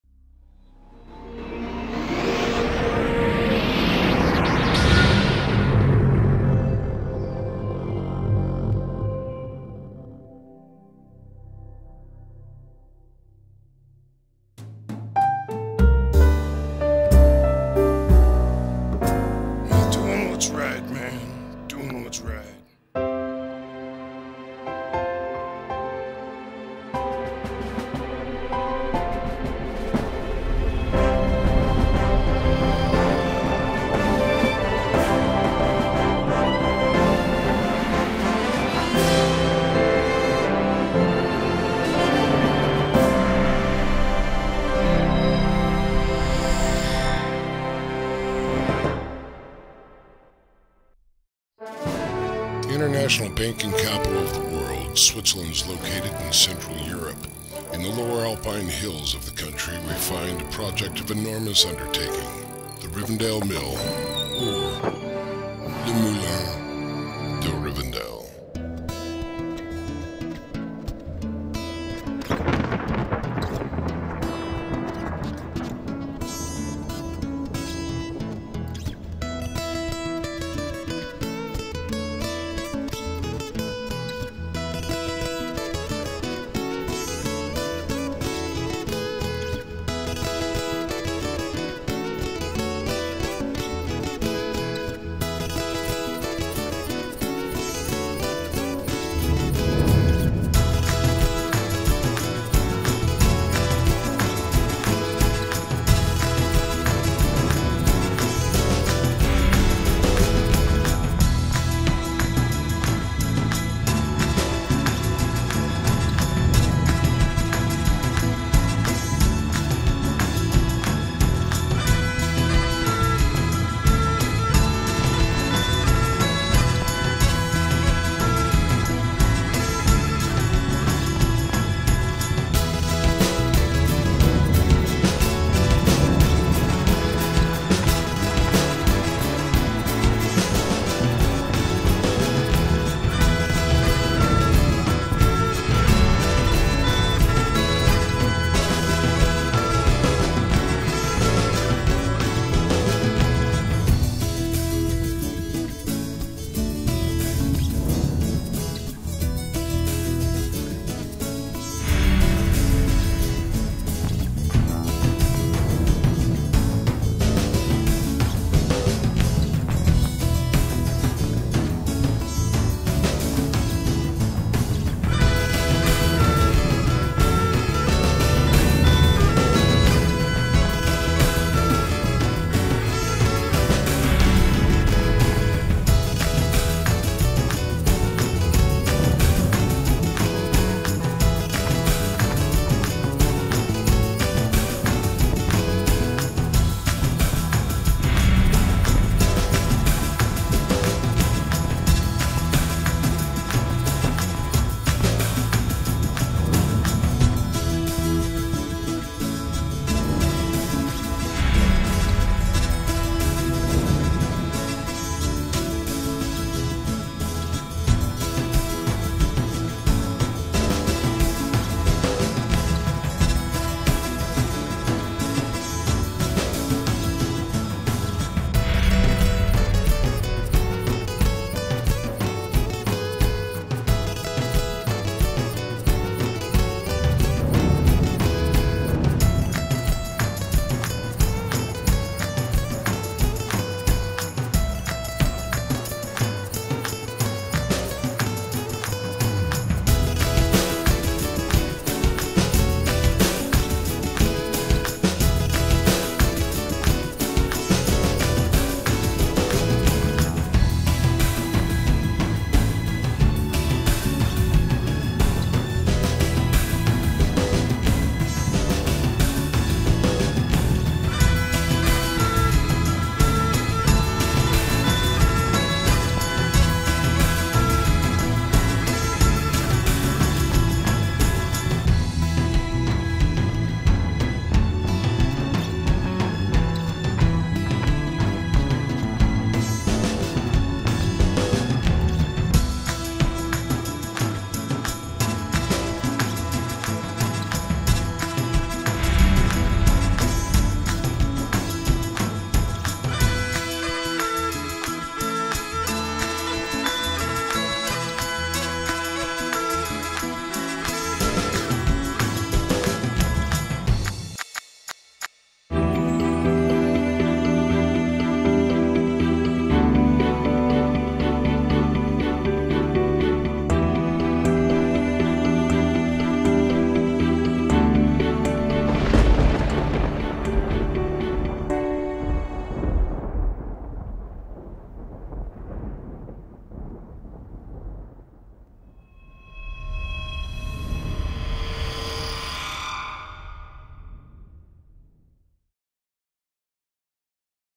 Rivendell Renovations: TOWING THE LINE-Renovation Blueprints by Duane Kemp
This is to show how the blueprints were made and from what shots. The print version of these house plans a 1/50th scale and will continue to be adapted as the house metamorphoses.